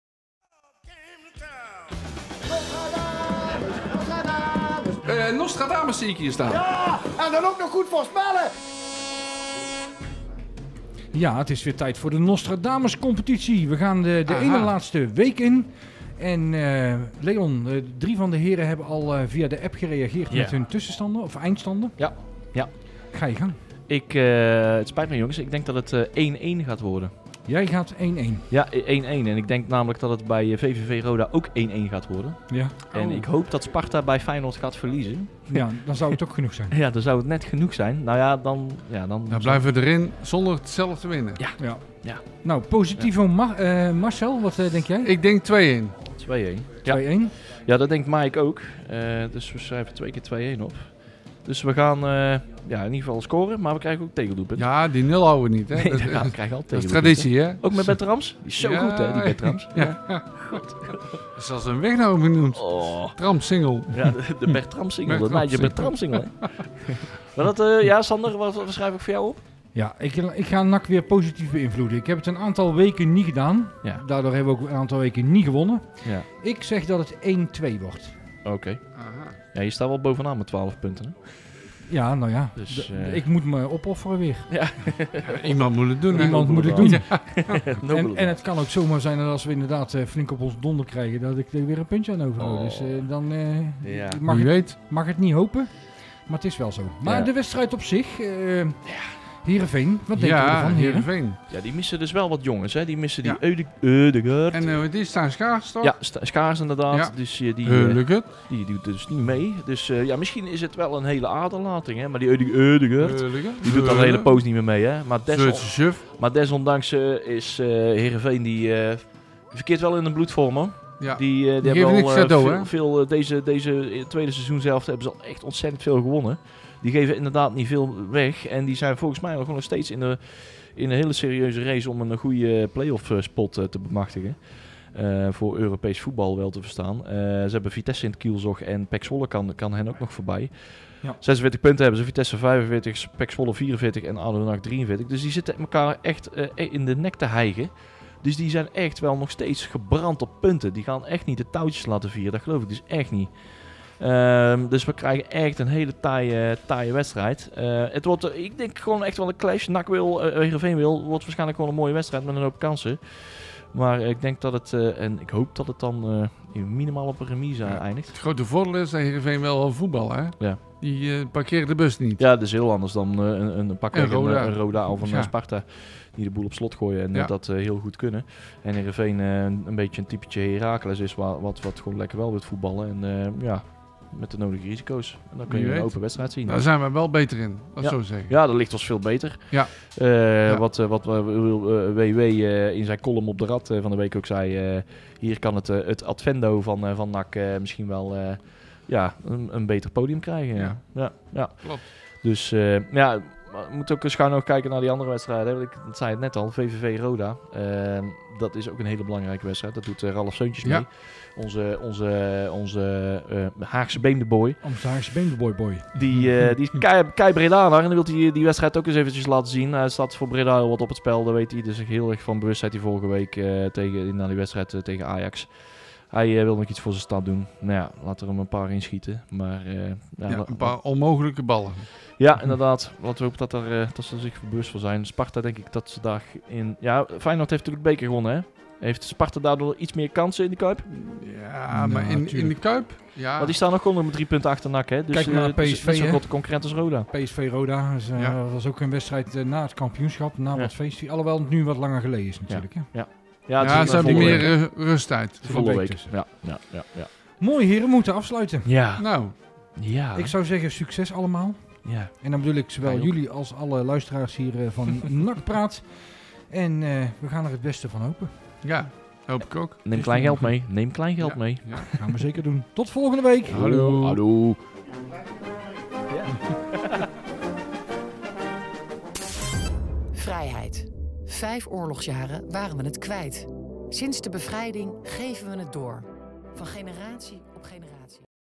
NACpraat 26 4 2018 Vooruitblik NAC - Heerenveen
De vooruitblik naar de wedstrijd van zondag: NAC - Heerenveen.